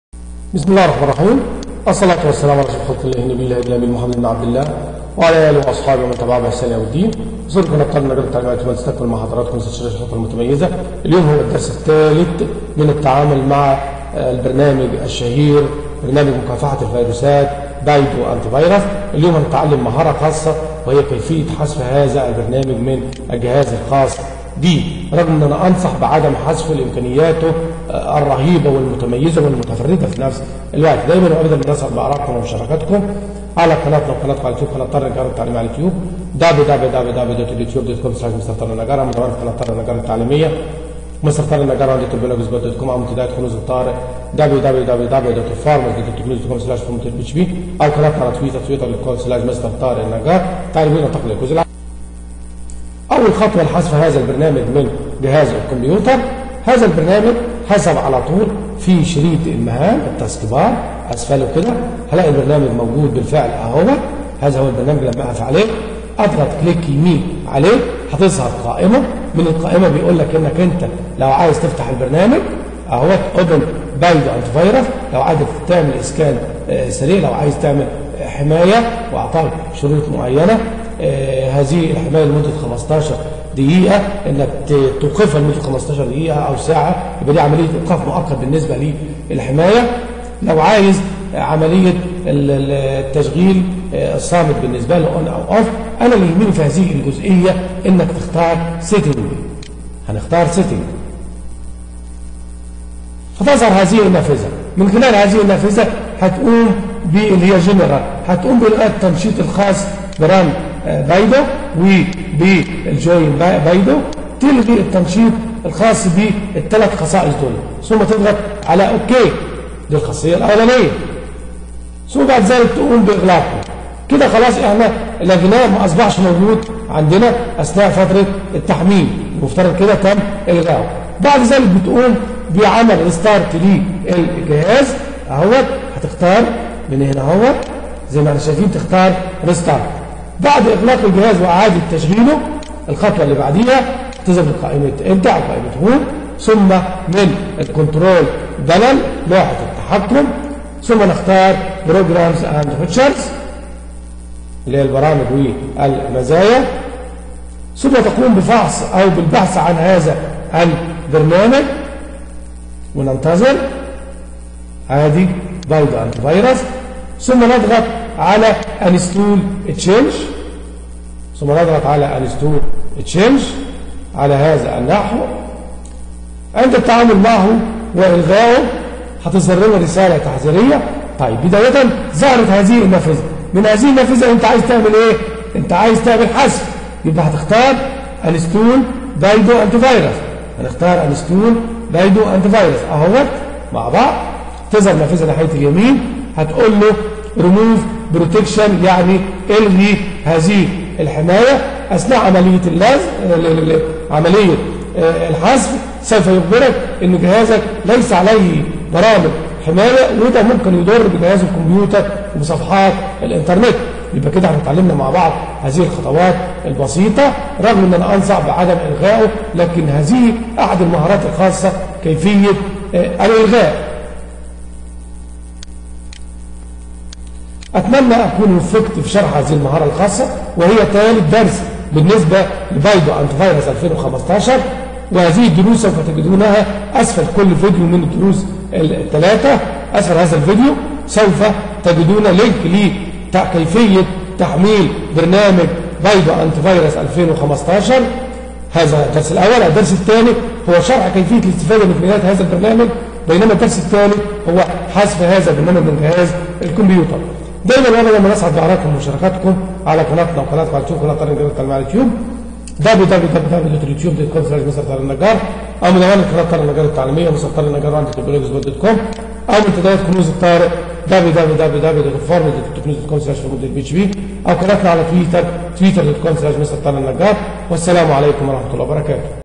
شرح طريقة حذف برنامج الحماية من الفيروسات Baidu AntiVirus 2015 من جهازك بطريقة صحيحة
يتناول هذا الفيديو شرح خطوات حذف برنامج الحماية من الفيروسات Baidu Antivirus 2015 بطريقة صحيحة و آمنة
لمتابعة باقي شروحات البرنامج اضغط على الروابط التالية :-